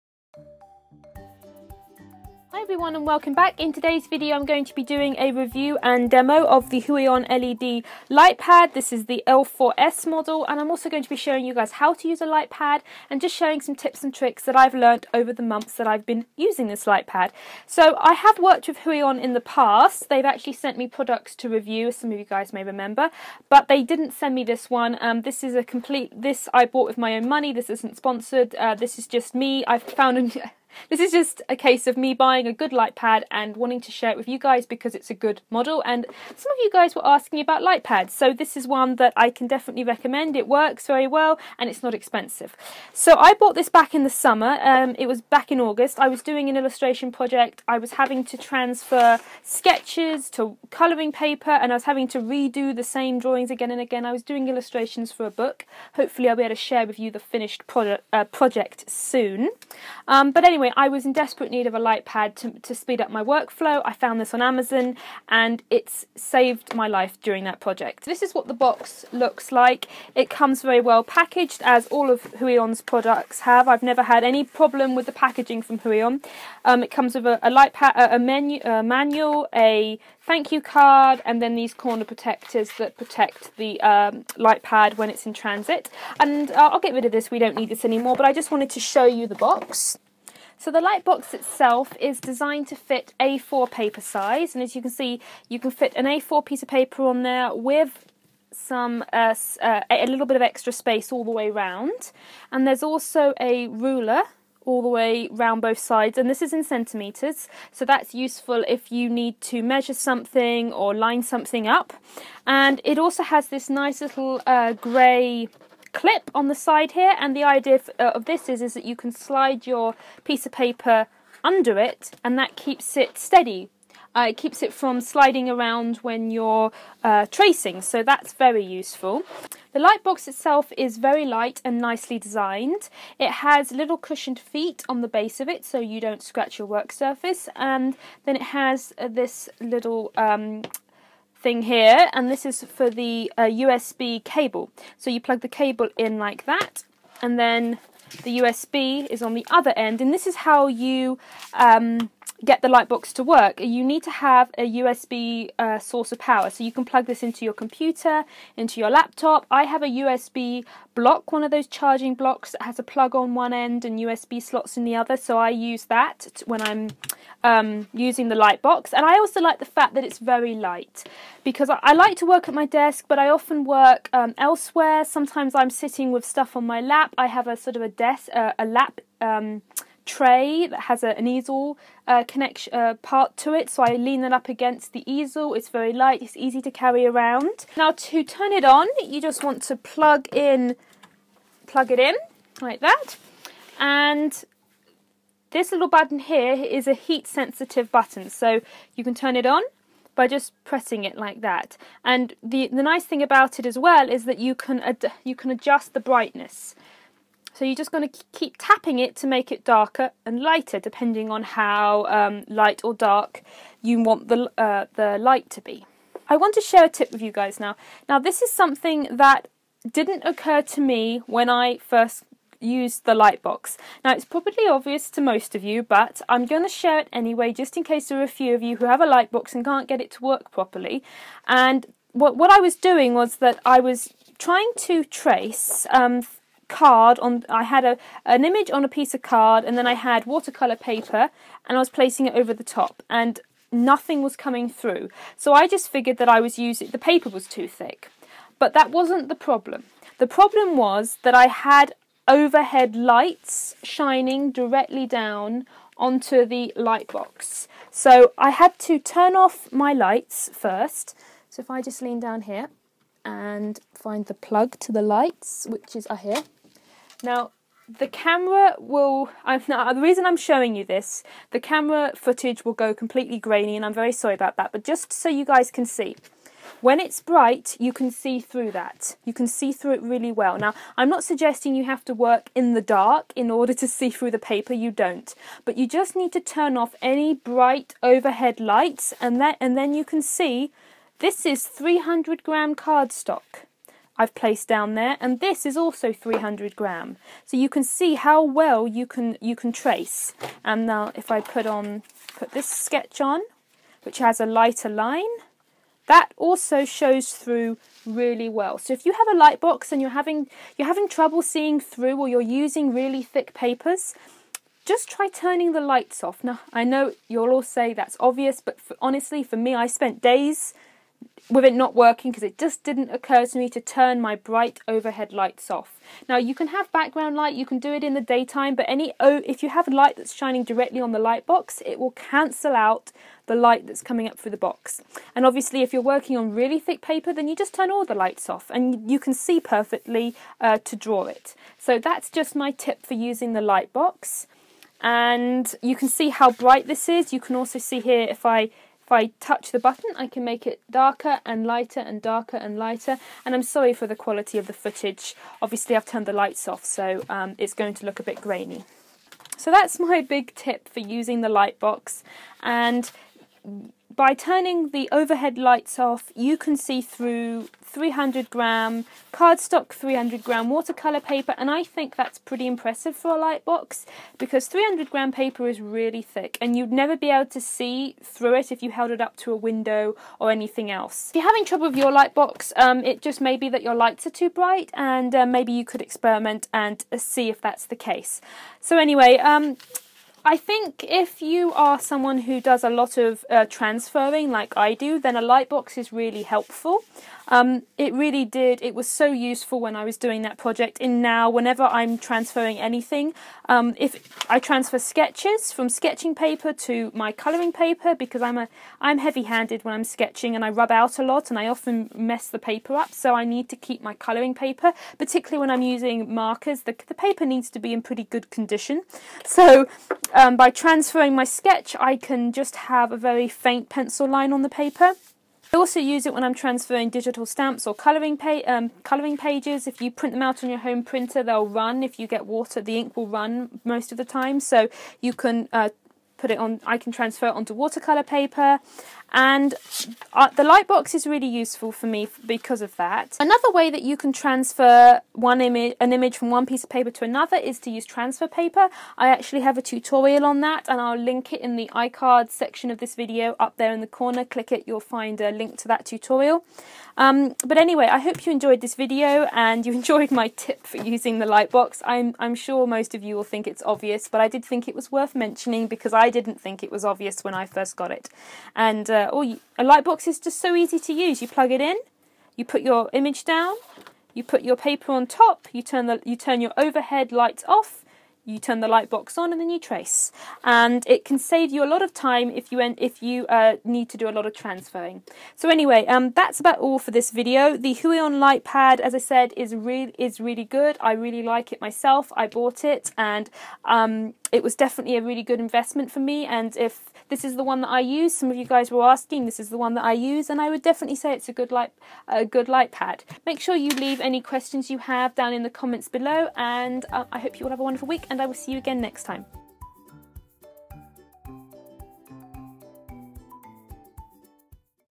How to Use a Light Pad or Light Box + Huion Light Pad Demo and Review
Today I'm going to be talking about how to use a light pad or light box to transfer or trace your images.Plus I am doing a small demo and review of the one I use from Huion. I also want to share a very important and (maybe obvious) tip that I learnt about using light pads to transfer your images from one piece of paper to another.

Supplies Used:
Graphite Pencil
Watercolor Paper
Huion Light Pad

Music Credit